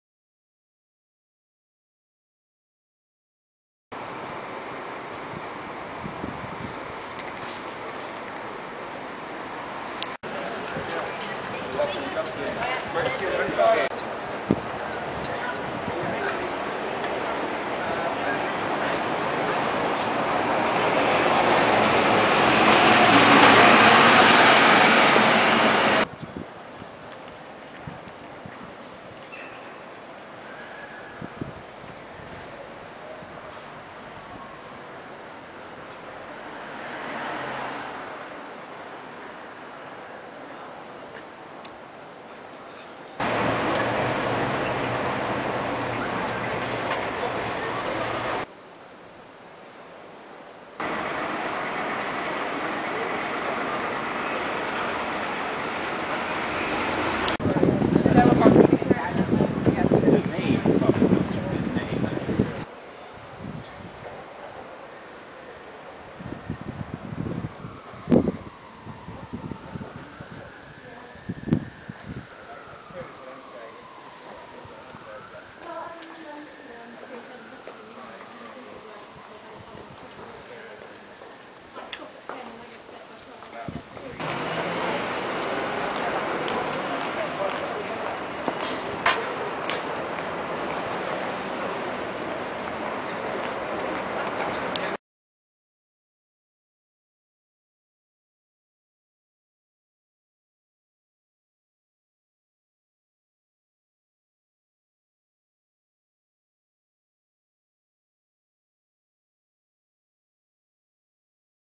GARGOYLES OF OXFORD
An architectural diversion, one lunchtime. Few quick shots of the gargoyles of Oxford that glare down mostly on the backs of people's heads.